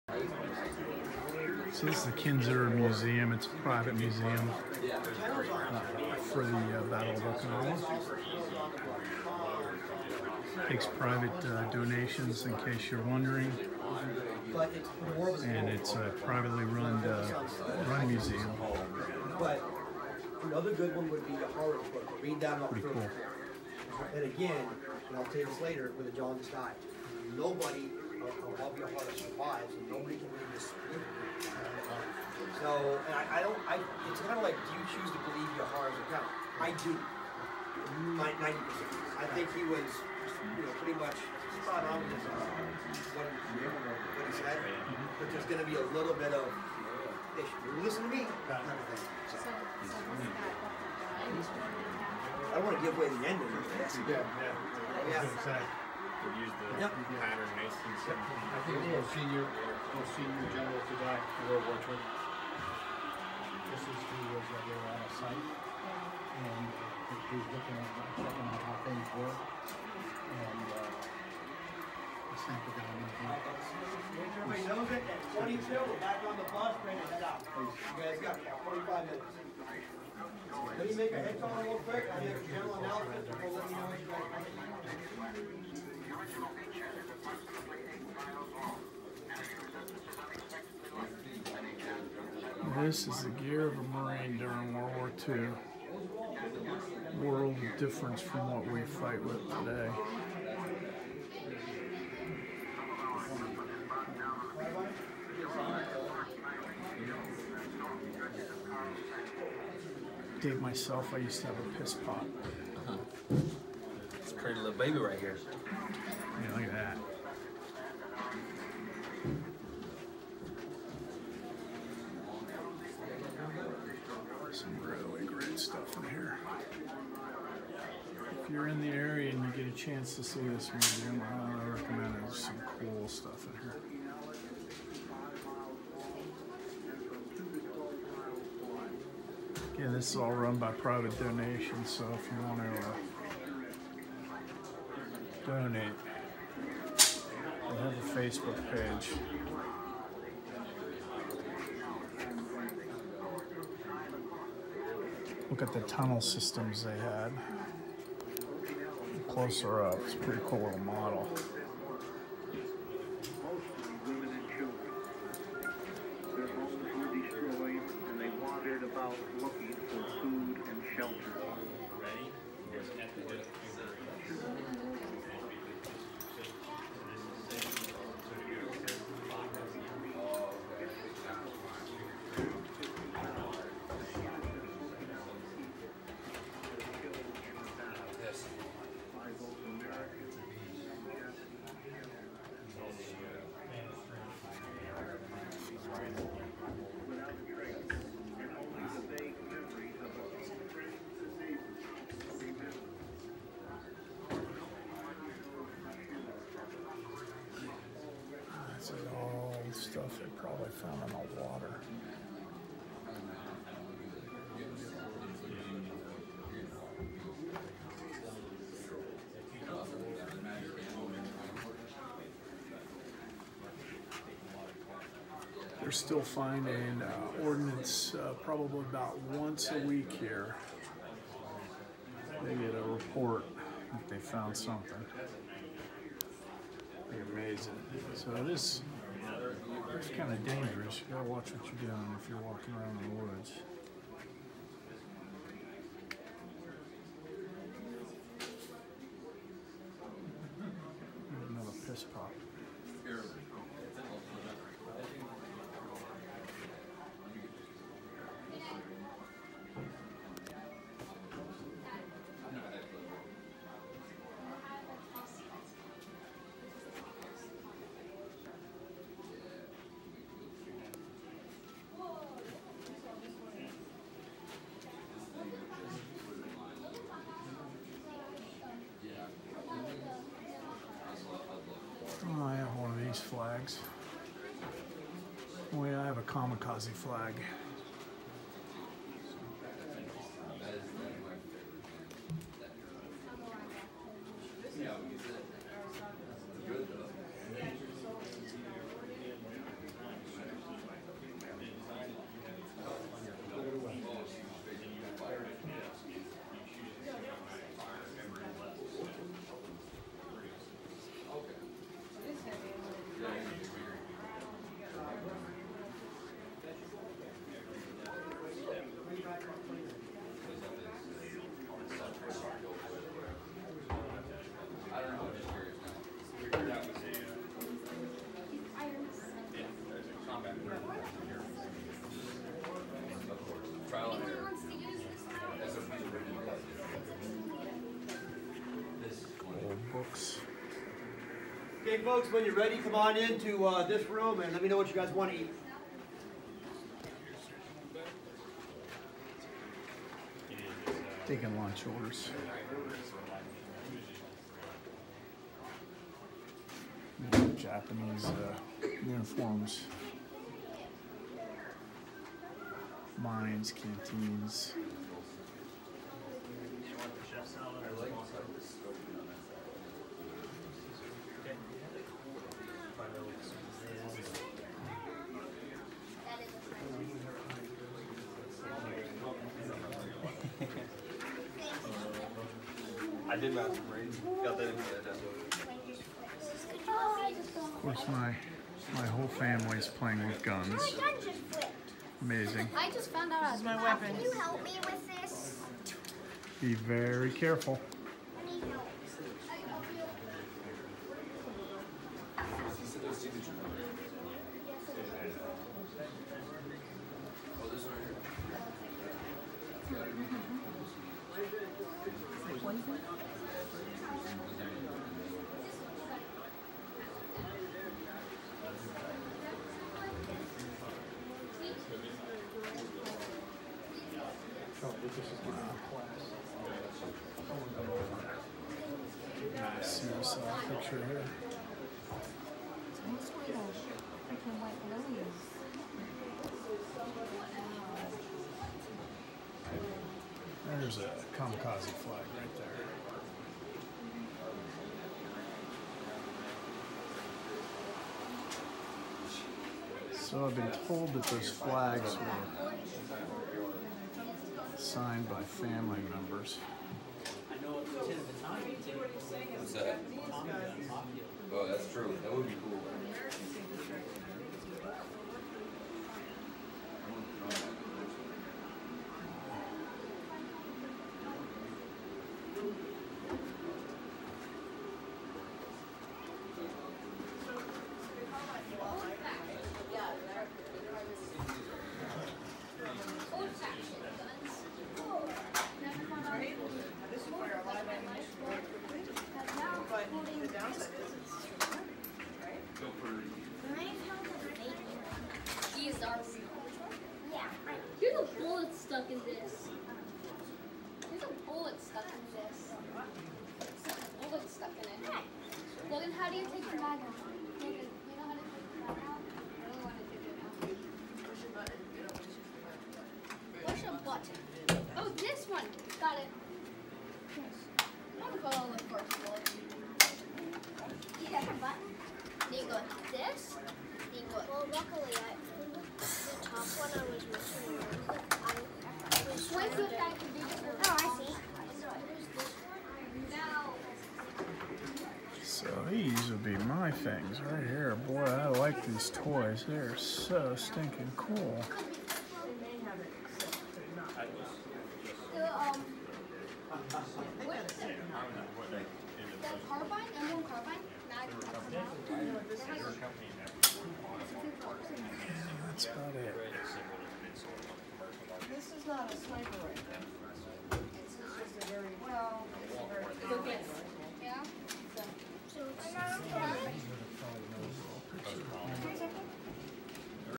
Exclusive, Private Tour of Battle of Okinawa Historical Society Museum on Camp Kinser, Okinawa
Located on Camp Kinser, the Battle of Okinawa Historical Display houses a fascinating collection of artifacts, photos, and memorabilia.

The museum is a volunteer only operation and maintains its collection from pieces donated through the generosity of veterans and private collectors, items recovered from construction sites and places of historical interest, as well items discovered and purchased on action web sites.  Here you will see newspaper and magazine clippings detailing frank accounts of the war, riveting photographs, personal effects from both American and Japanese soldiers, uniforms, rifles, swords, flags, and many other items.

The display is divided into sections representing major Marine Corps, Navy, and Army events during the battle.